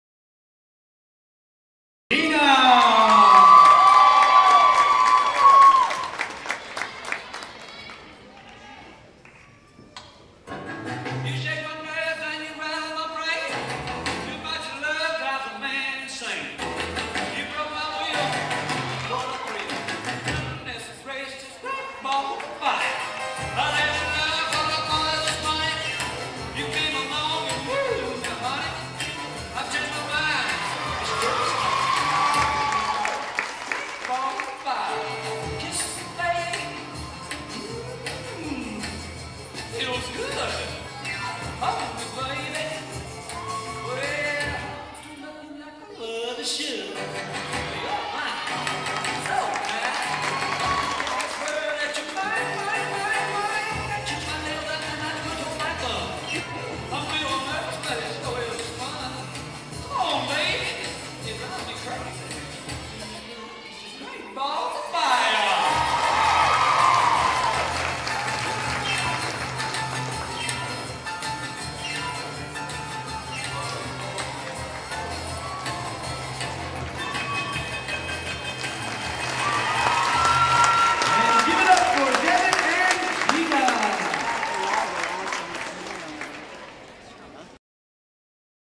Sherwood Devin Gina
Devin & Gina Individual Stunts competition @Sherwood H.S. Jan 5, 2013